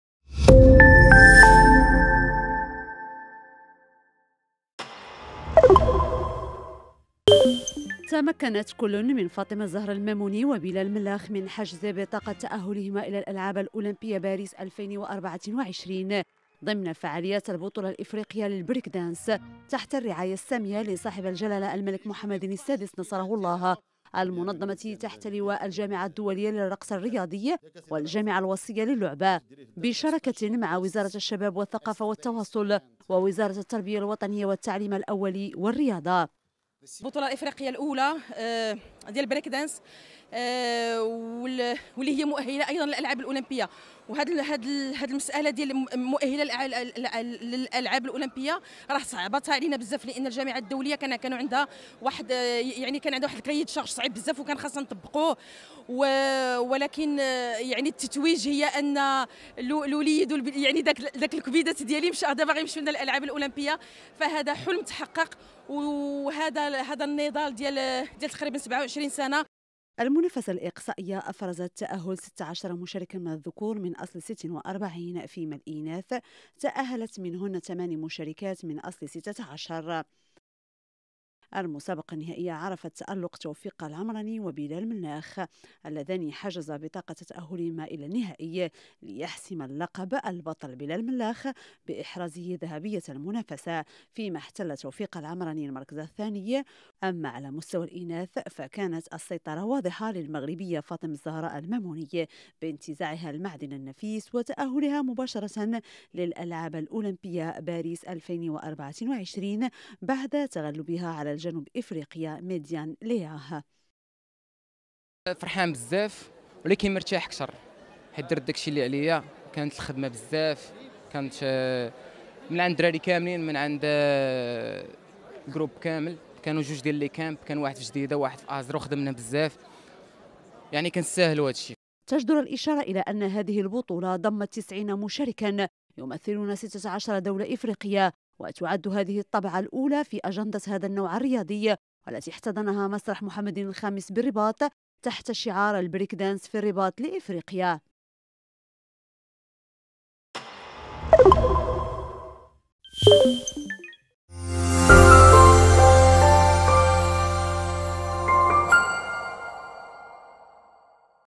روبورتاج قناة الرياضية عن التأهل التاريخي للمغرب إلى دورة الألعاب الأولمبية باريس 2024
روبورتاج قناة_الرياضية ليوم الأحد 14 ماي 2023، عن التأهل التاريخي للمغرب إلى دورة الألعاب الأولمبية باريس 2024 بعد التتويج باللقب الإفريقي في البريكين في صنفي الذكور والإناث وتحصيل الفريق الوطني المغربي لميداليتين ذهبيتين وميدالية فضية في أول بطولة إفريقية للبريكين "WDSF Breaking Continental Championships Africa" والتي جرت أطوارها على مدى يومي 12 و 13 ماي ٢٠٢٣ بمسرح محمد الخامس بالرباط.

متابعة طيبة